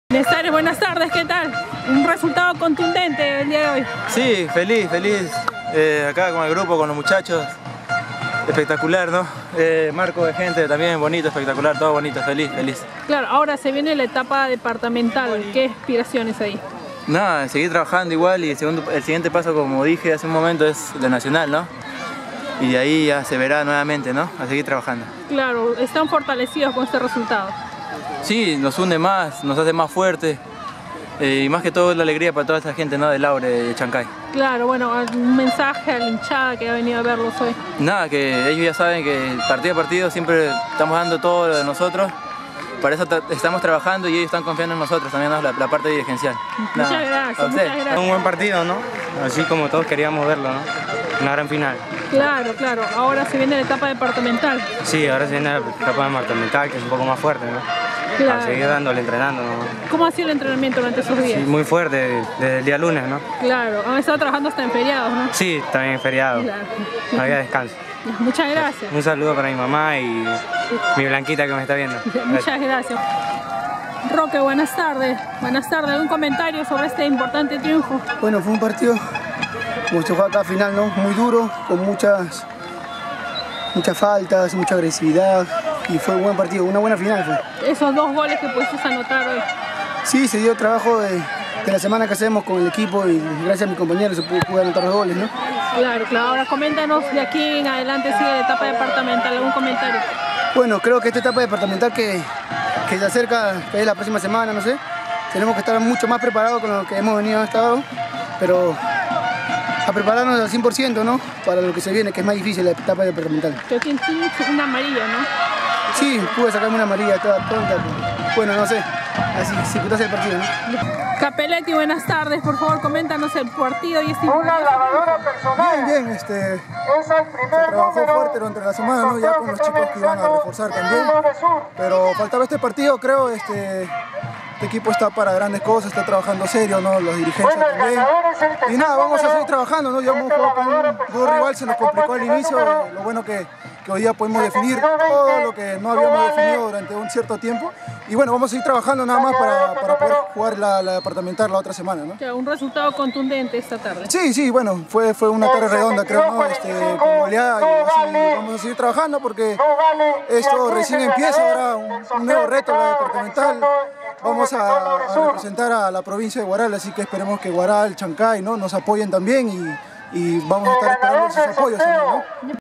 COPA PERÚ. Entrevista jugadores Laure Sur de Chancay
Entrevista a los jugadores del club Defensor Laure Sur de Chancay al final del partido con Social La Huaca de Huaral por el titulo provincial de la Copa Perú 2017.